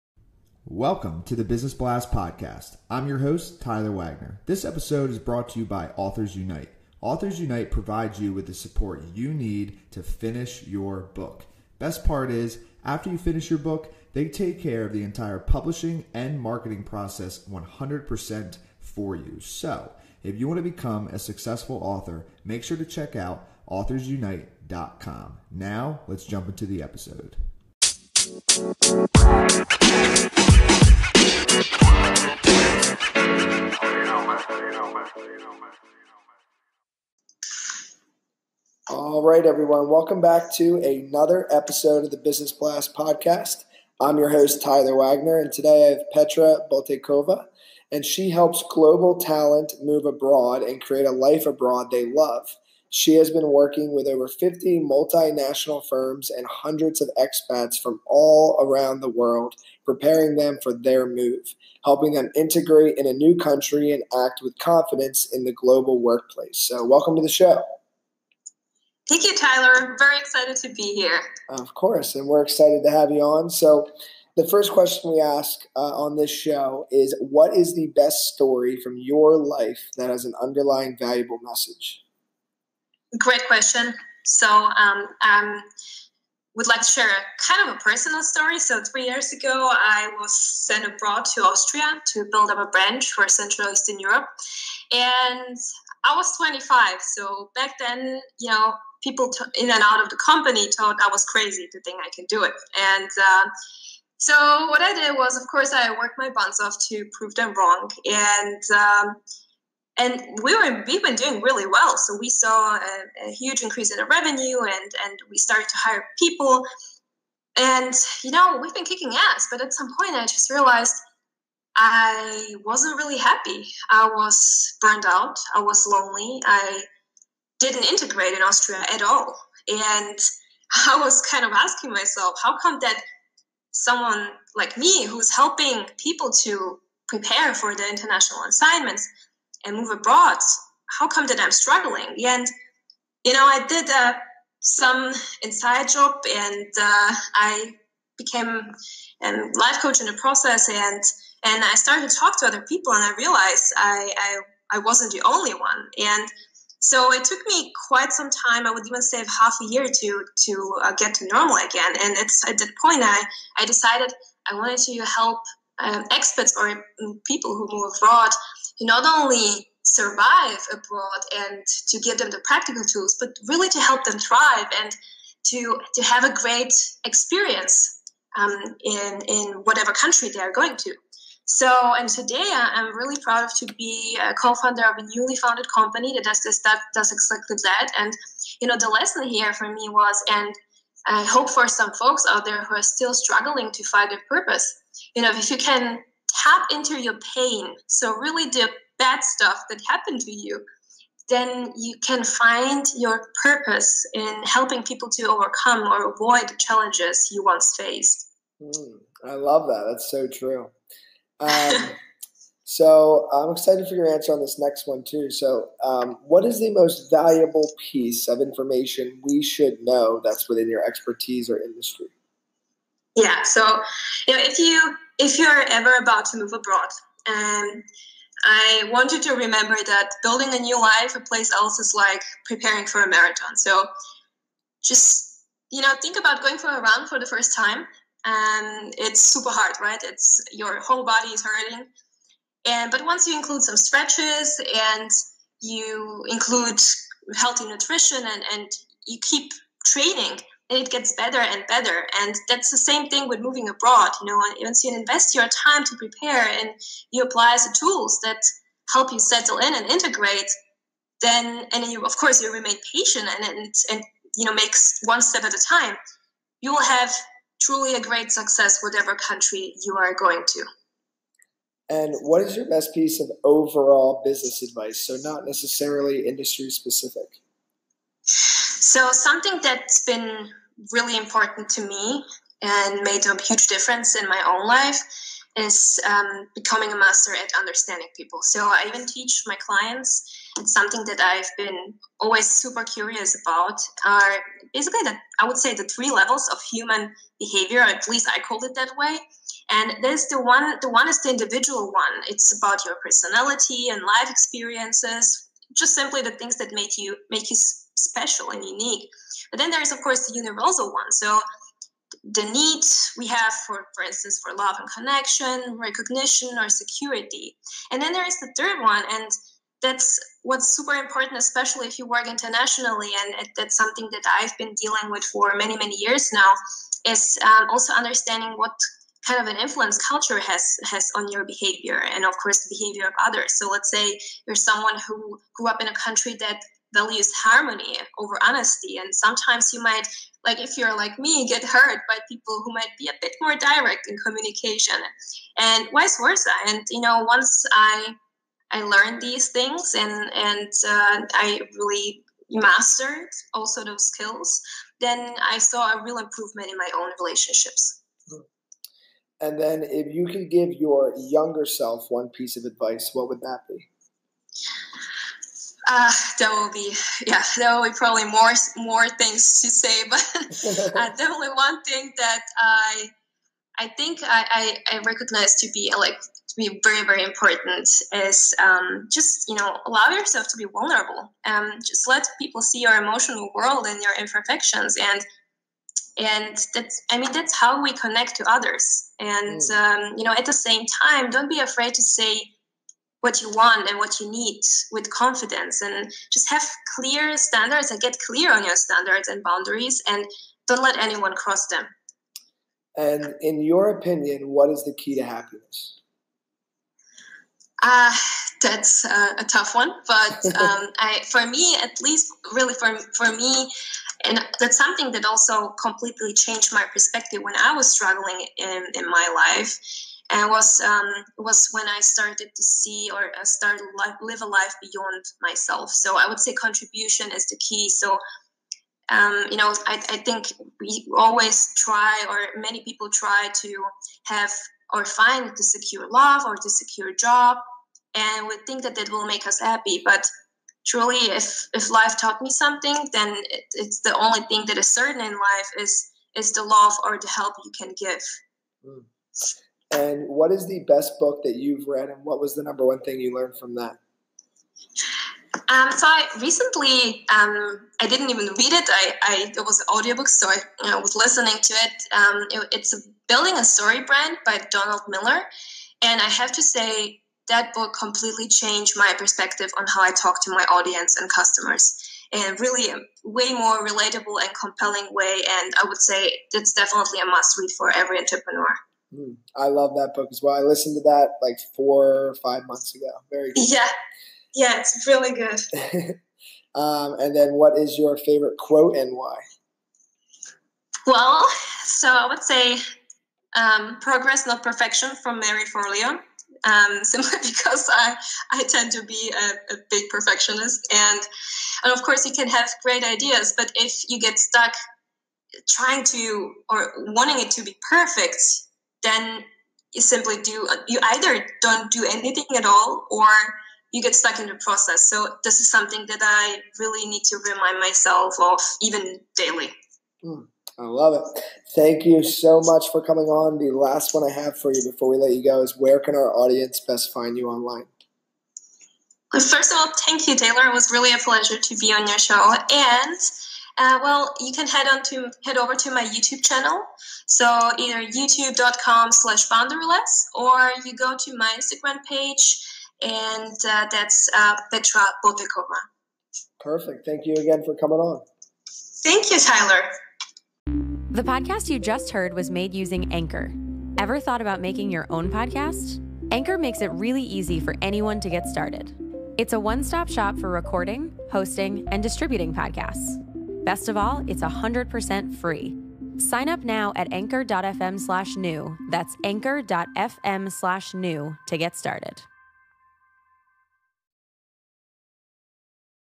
Petra Boteková: Create A Life Abroad You Love
Petra Boteková helps global talent move abroad and create a life abroad they love. She has been working with over fifty multinational firms and hundreds of expats from all around the world preparing them for their move, helping them integrate in a new country and act with confidence in the global workplace.

Tyler